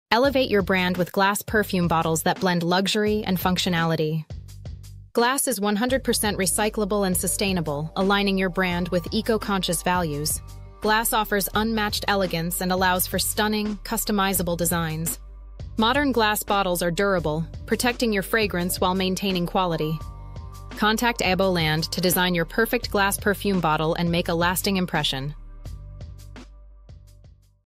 Elevate Your Brand with Glass Perfume Bottle Packaging
This video is mainly to show our products to you. Include perfumebottlesupplier customperfume bottle perfumebottlewholesale perfumesamplebox orangebottleperfume perfumediffuserbottle and so on.

In today's competitive market, first impressions are everything, and the packaging of your product plays a significant role. This is especially true in the perfume industry, where the bottle is often as important as the scent itself. In this post, we will explore why glass perfume bottle packaging is the best choice for brands looking to stand out.